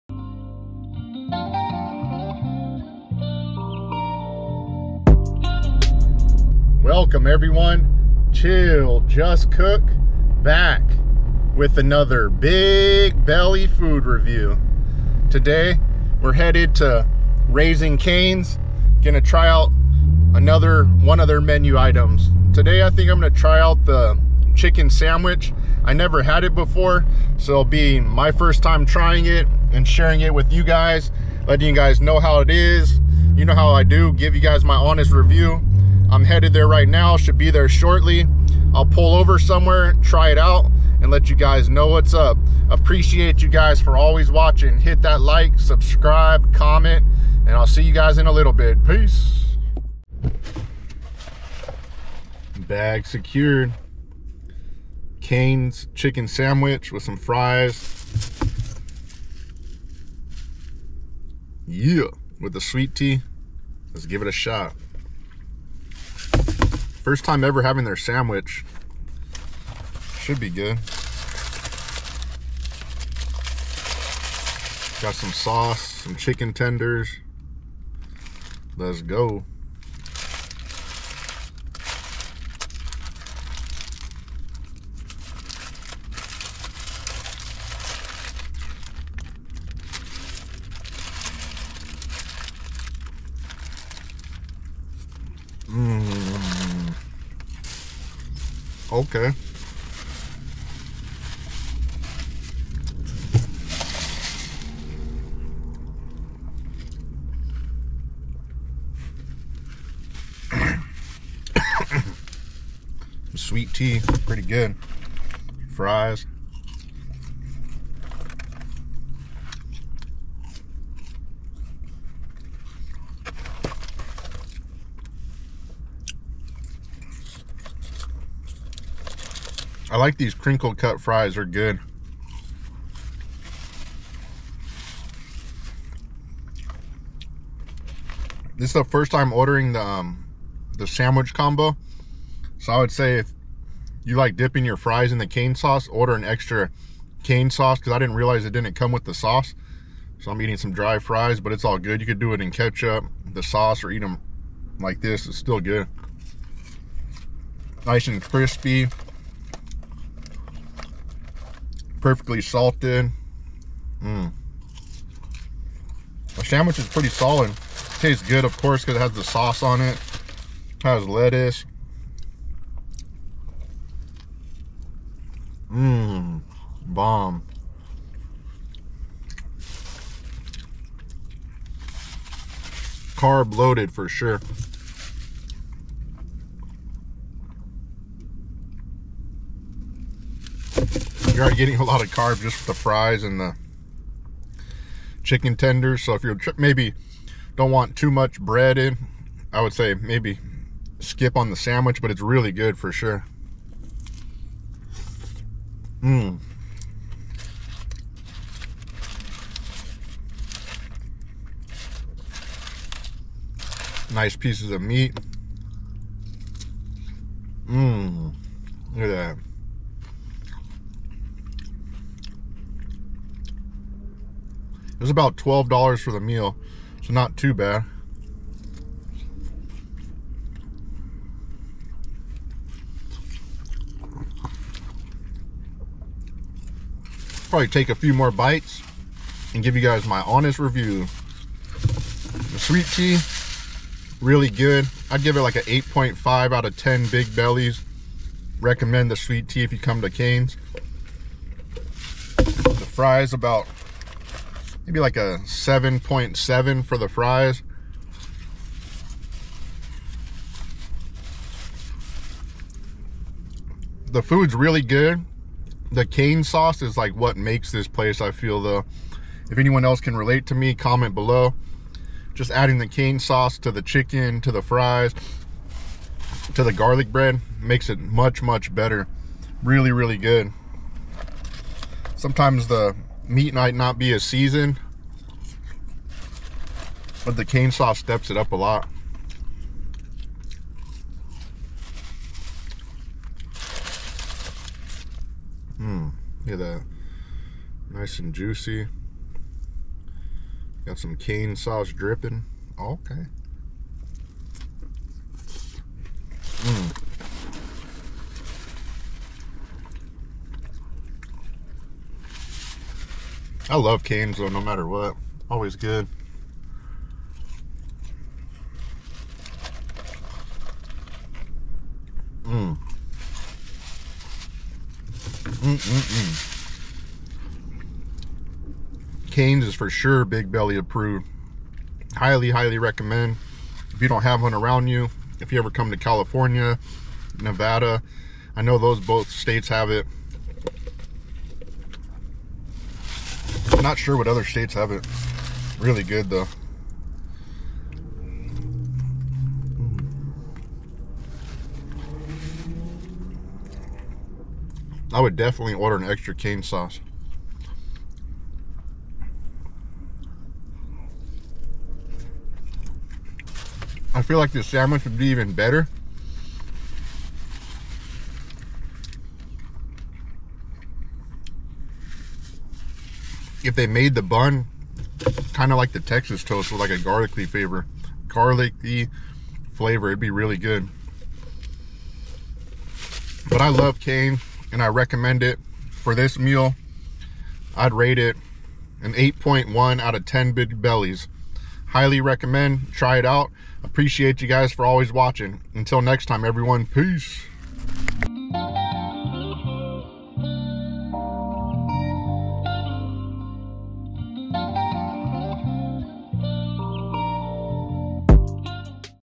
Raising Cane's SANDWICH COMBO Review (Big Belly Food Review)
Today on this amazing review for the first time I tried out the chicken sandwich from Raising Cane's. The sandwich was good and id recommend trying it out.